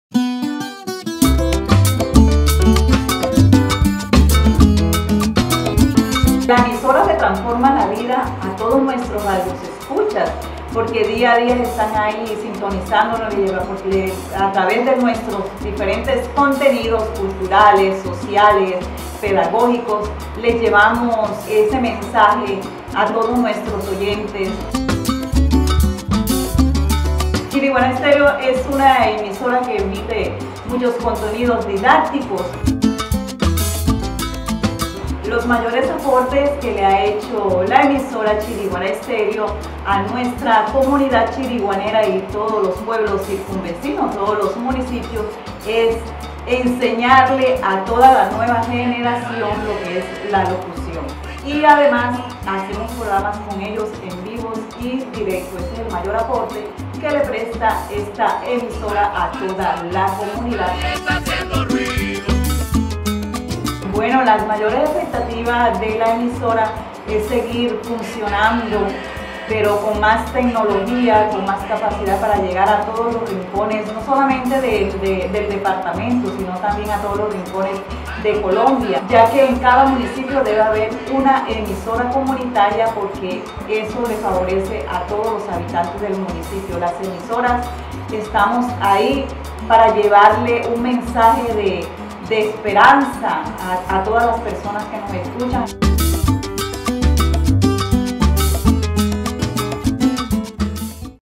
Loly Bustamante. Radio Comunitaria Chiriguaná Estéreo. Chiriguaná, César.
Chiriguaná Estéreo es una emisora que emite muchos contenidos didácticos.